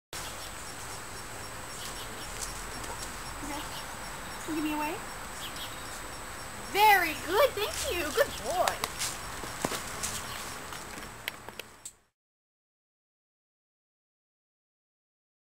Tiffany and Capone attempt 'WAVE'
For Chazhound's Trick Train-off... (Sorry about the quality... my first upload, so hopefully they'll improve.)

The backyard here seems to be super-distracting for him... critters abound. (Please note: Capone is a cherished family member, not a white trash lawn ornament. He's only on his tieout for 'potty time' and training with distractions. He is *never* out there without me.)